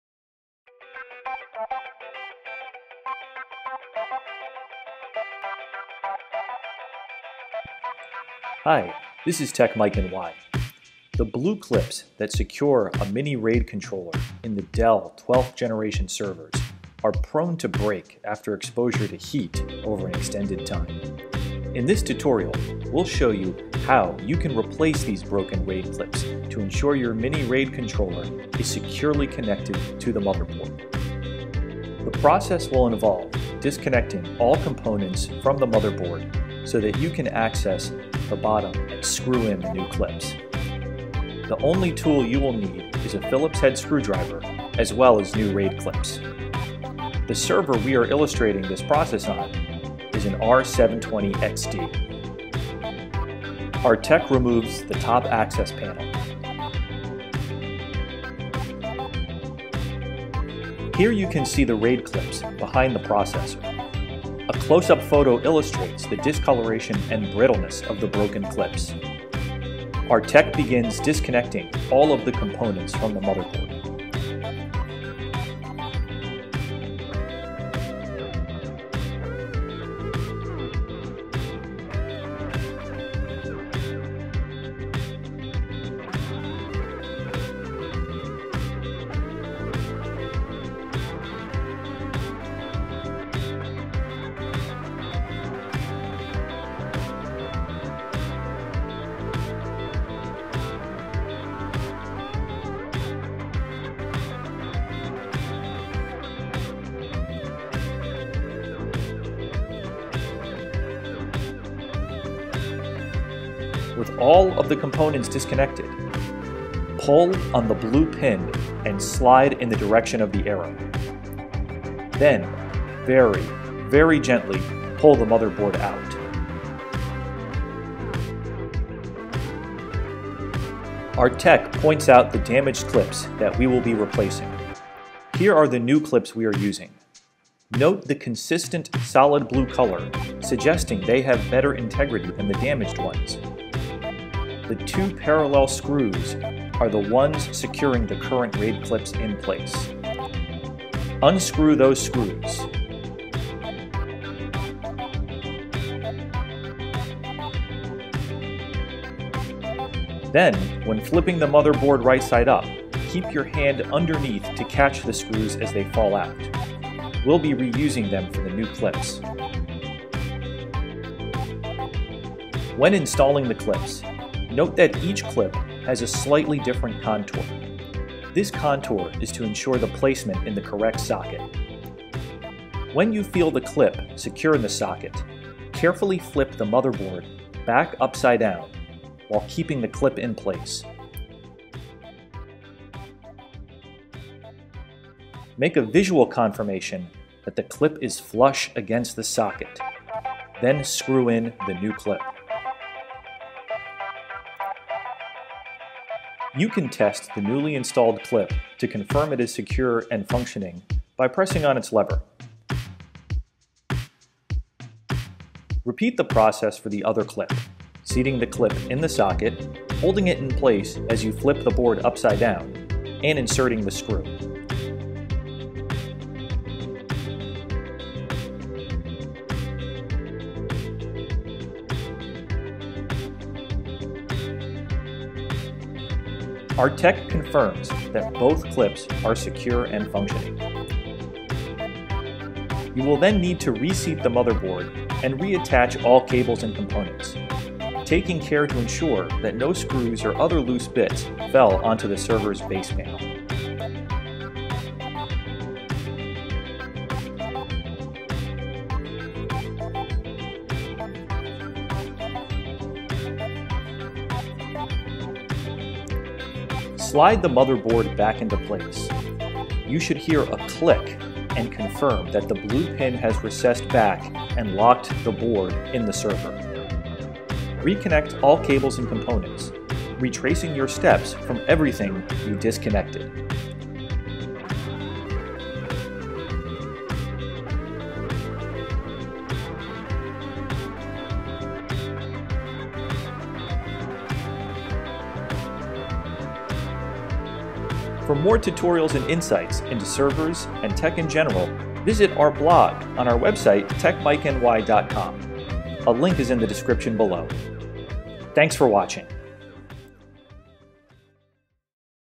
Replacing Broken RAID Clips in Dell 12th Gen Server
This tutorial shows you how to replace the blue RAID clips in Dell 12th Gen Servers.